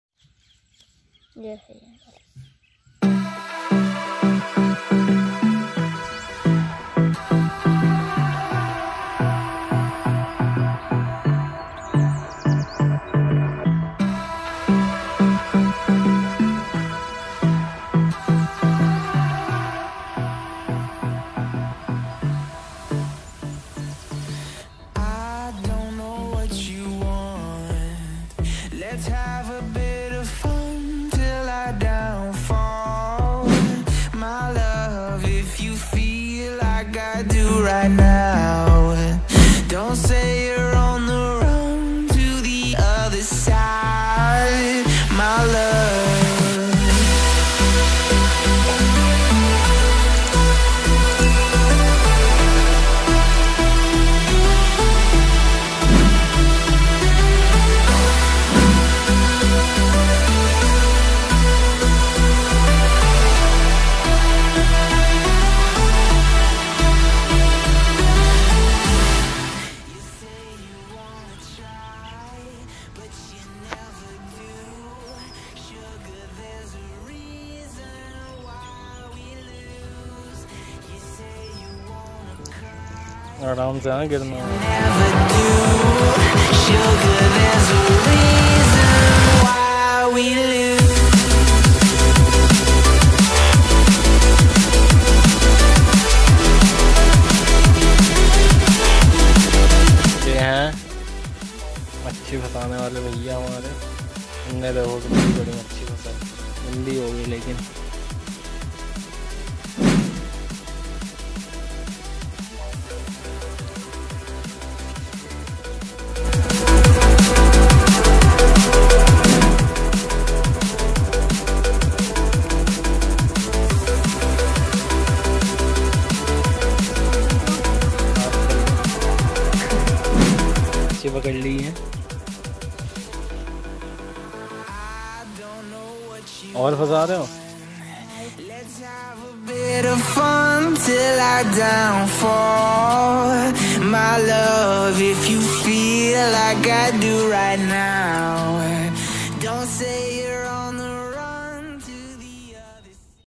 Aaj toh ! Full fish curry , How to Fishing in river , Little boy fishing
Amazing Hand Fishing Video | Traditional Boy Catching Fish By Flour in Pond Water
Aaj Toh ! Bhiya Ki Fish 🍛 Curry Pakka Hai
Try To See The Good Thing Machli Jal Ki Rani hai

Amazinghandfishing